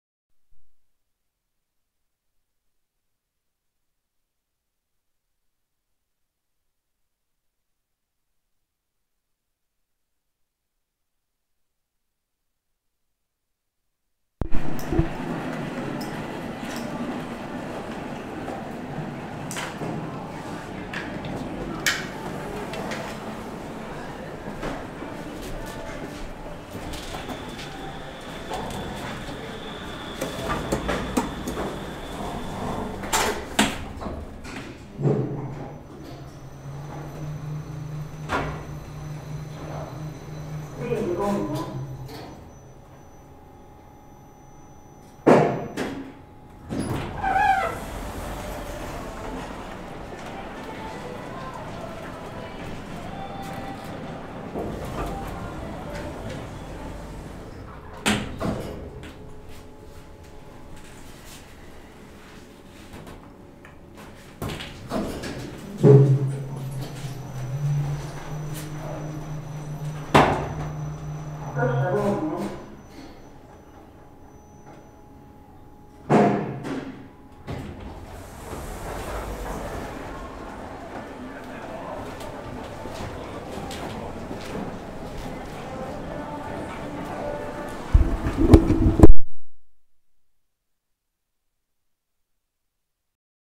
2010 KONE MonoSpace MRL-Traction Elevator @ Åkersberga Centrum
ELEVATOR INFORMATION!

Manufacturer/Tillverkare: KONE Hissar AB
Model/Modell: MonoSpace
Installation year/Byggår: 2010
Capacity/Maxvikt: 1600Kg - 21 Persons
Floors/Antal våningar: 3 = (0) 1 3

Location/Adress: Åkersberga Centrum | Stationsvägen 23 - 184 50 Åkersberga Sweden!